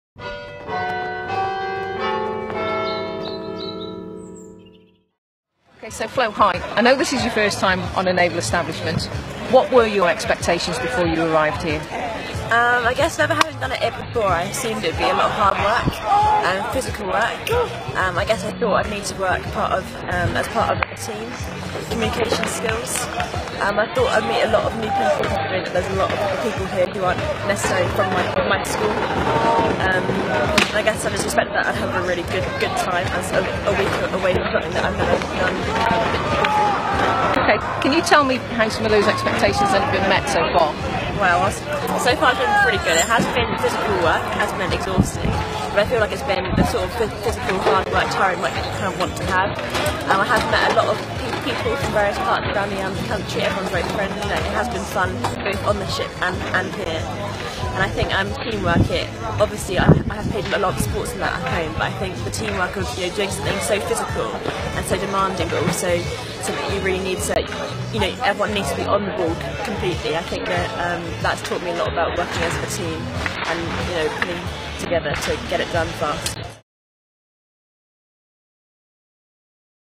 Junior Leaders Field Gun Competition 2018 Interview
Winners of the Endeavour Trophy at this year's Junior Leaders Field Gun Competition.